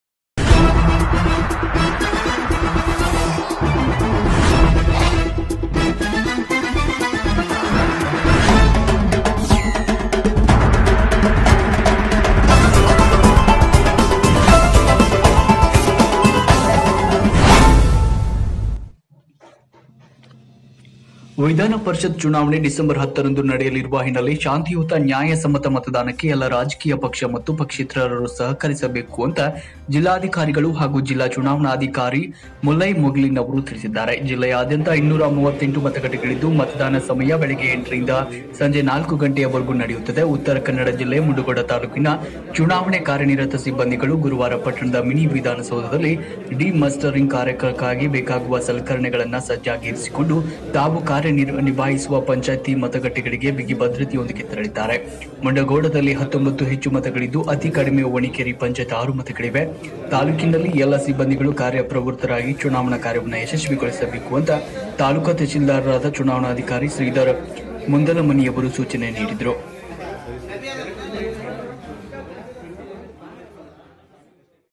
ಮುಂಡಗೋಡ: ವಿಧಾನ ಪರಿಷತ್ ಚುನಾವಣೆ ಡಿ.10 ರಂದು ನಡೆಯಲಿರುವ ಹಿನ್ನಲೆ ಶಾಂತಿಯುತ ನ್ಯಾಯ ಸಮ್ಮತ್ ಮತದಾನಕ್ಕೆ
Video from Agsty